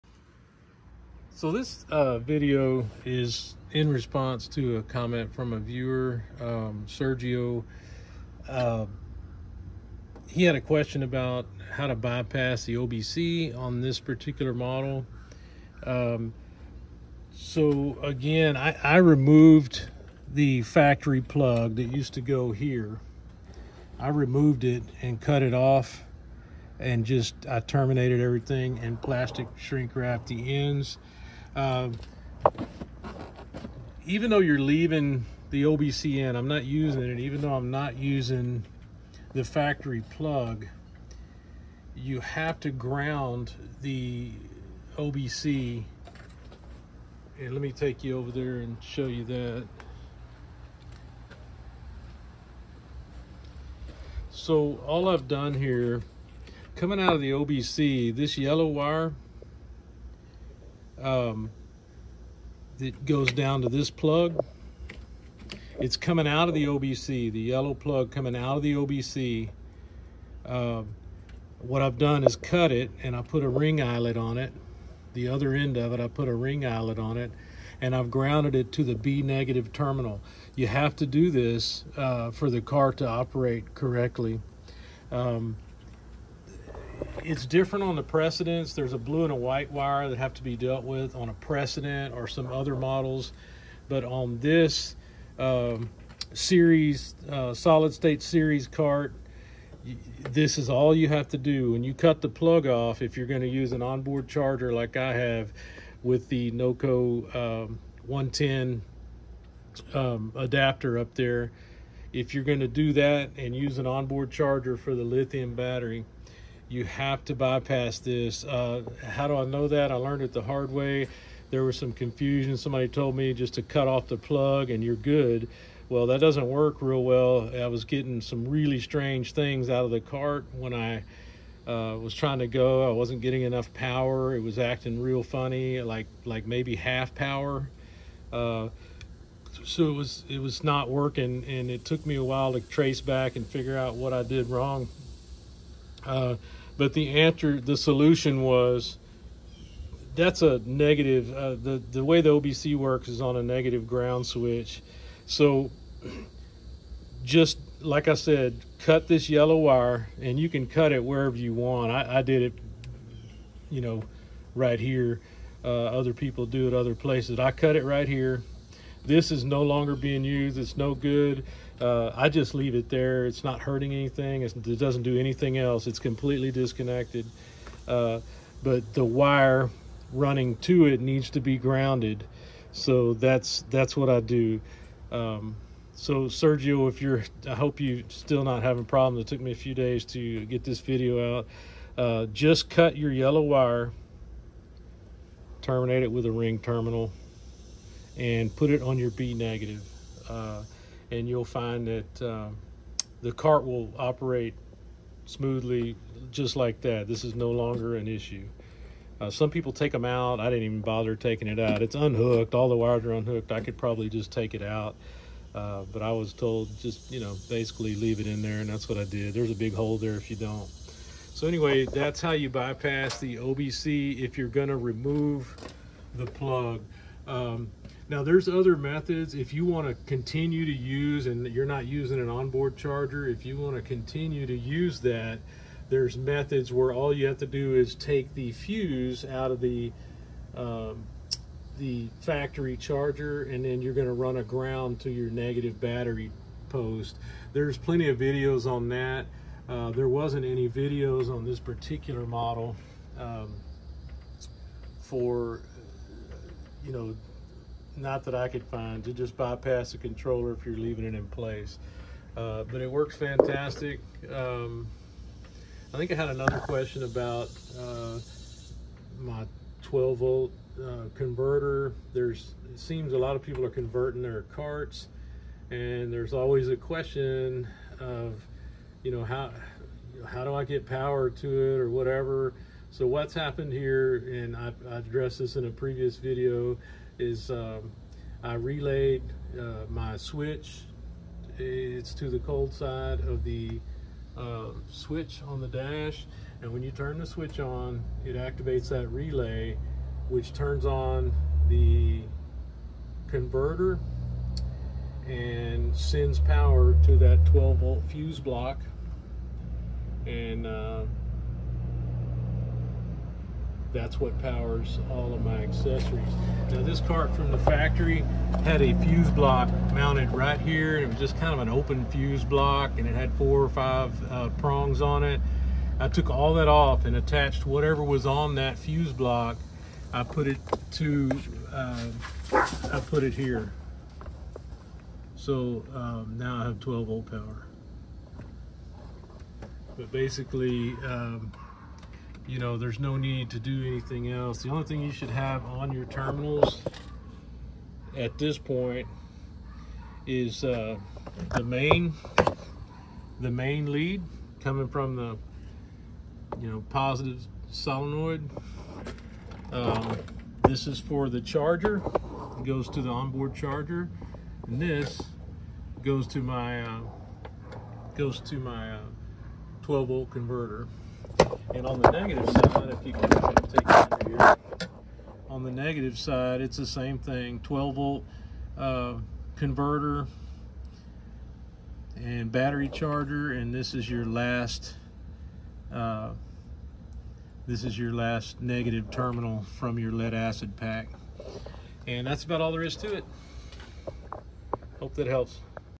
Club Car 2001 48v OBC bypass
Quick video to answer questions related to removing factory charge port to replace with onboard charger and bypass OBC